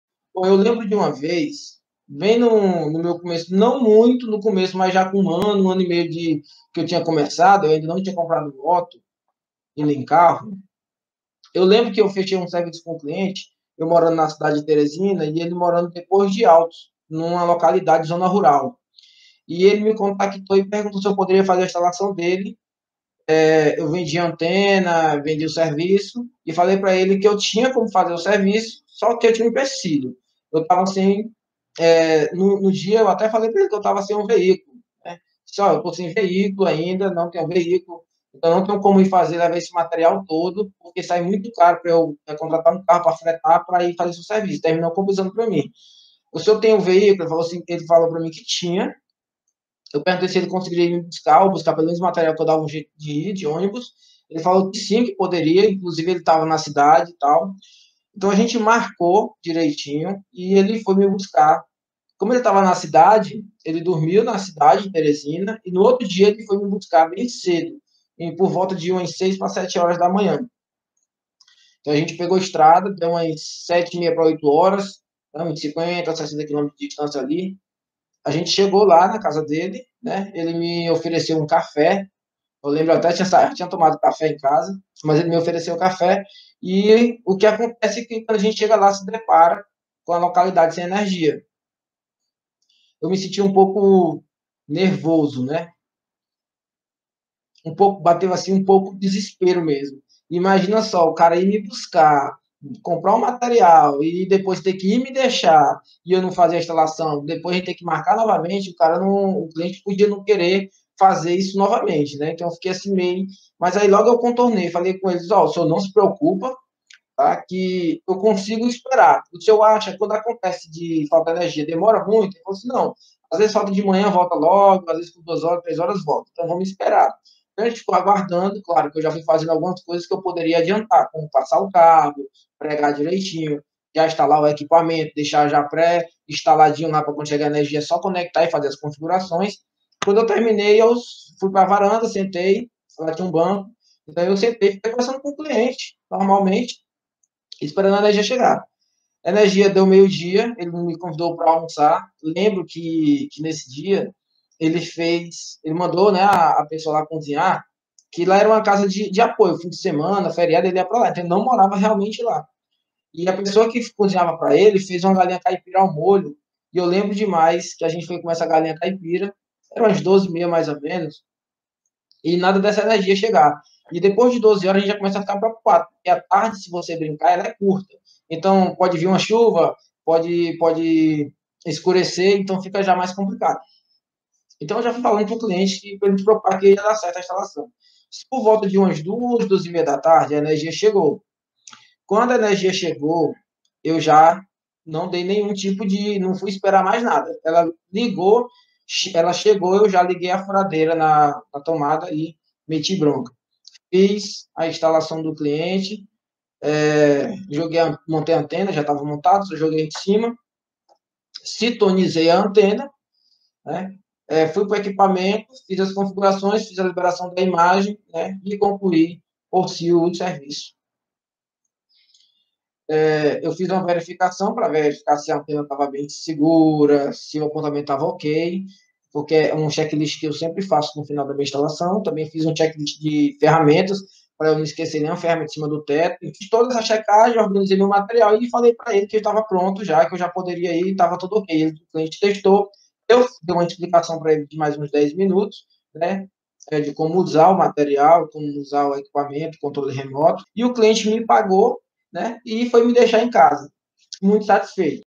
Como trabalhar com antena banda ku sem ter carro e nem moto
Nesse video conto uma historia de como eu fazia meus atendimento sem tere carro ou moto.